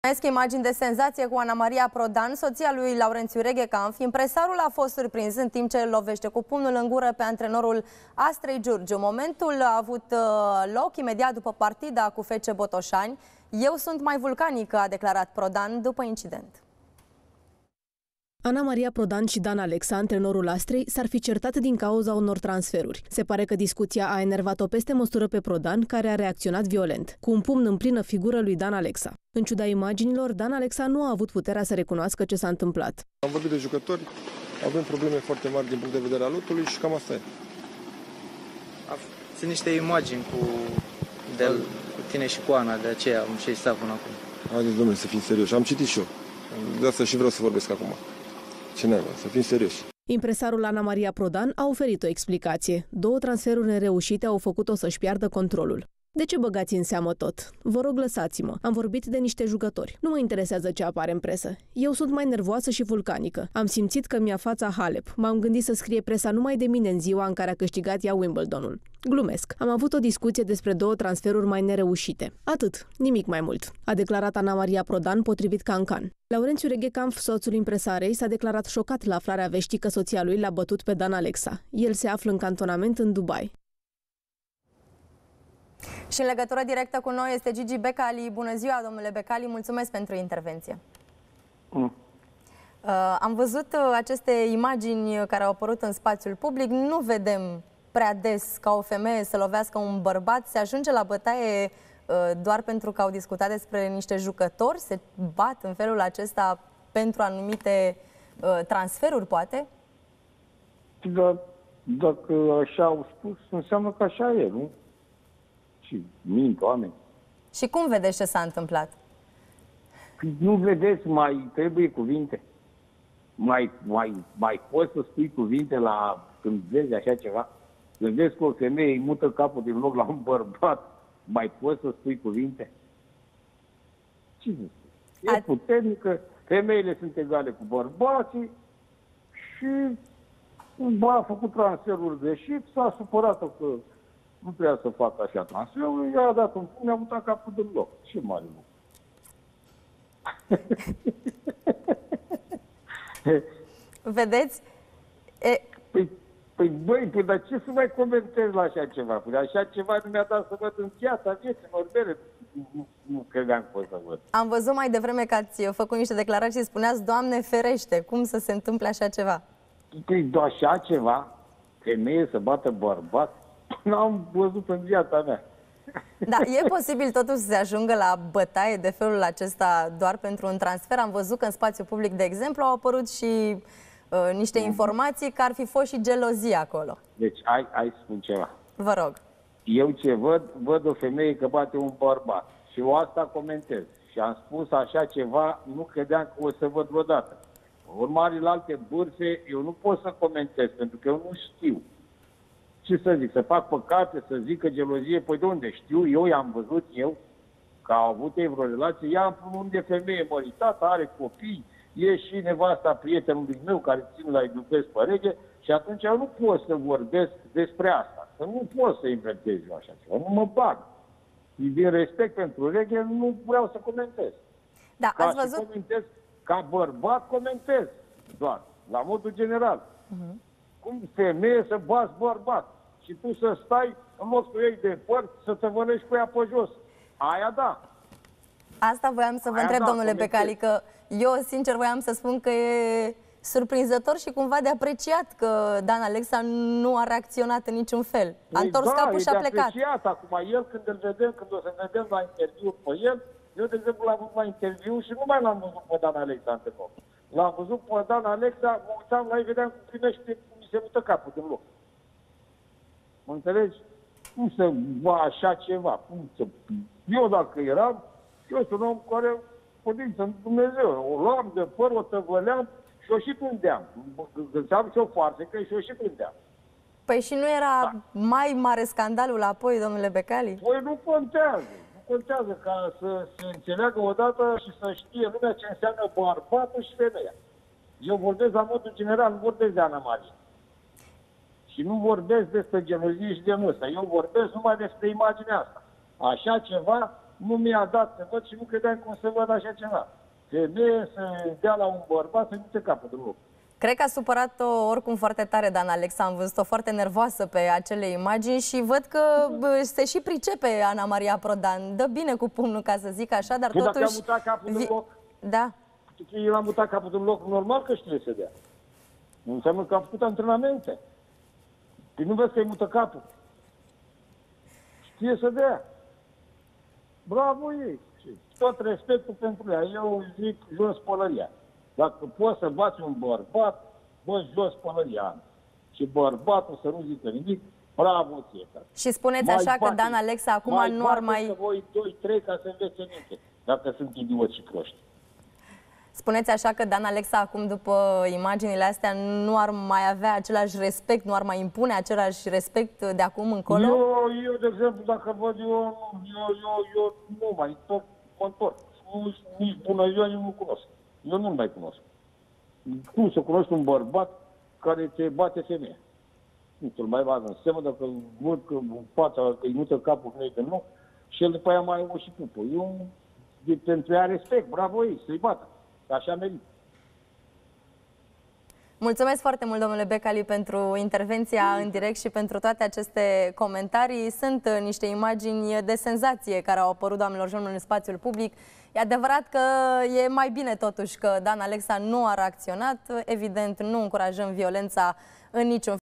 Gigi Becali, reacție spumoasă după ce Anamaria Prodan l-a lovit pe Dan Alexa
Gigi Becali, reacție spumoasă după ce Anamaria Prodan l-a lovit pe Dan Alexa: Eu dacă eram, o luam de păr o tăvăleam și o și tundeam.